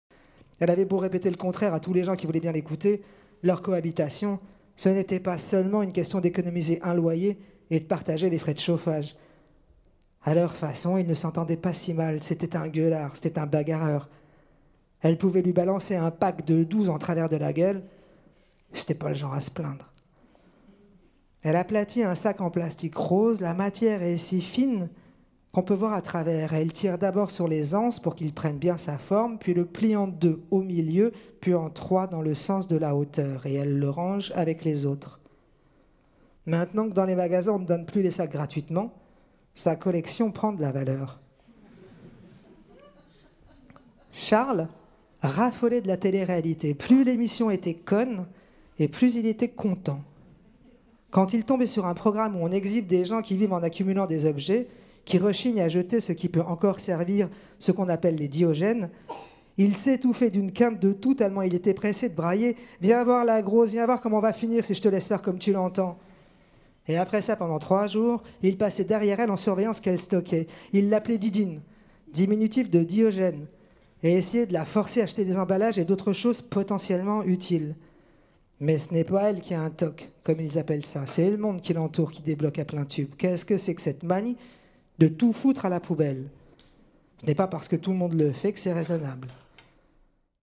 Virginie Despentes "Vernon Subutex III" 01/06/17 Paris, Maison de la Poésie (3/10)
lecture